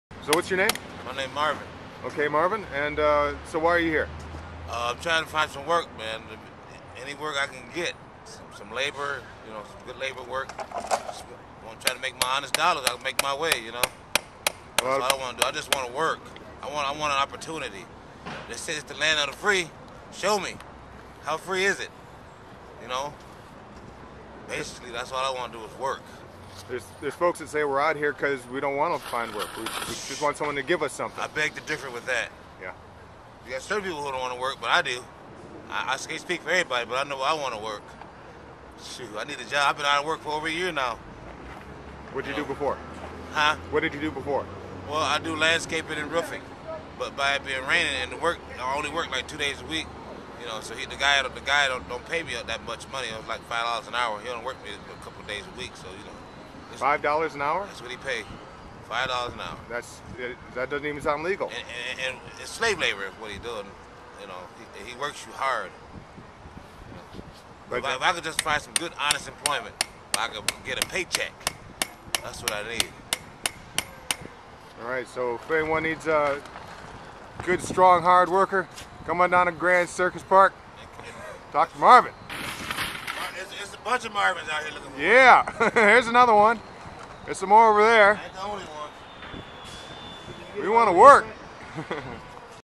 Call MARVIN
Herman Cain says protesters such as the ones Occupying Detroit are not trying to find work.  I'd like you to meet one of them.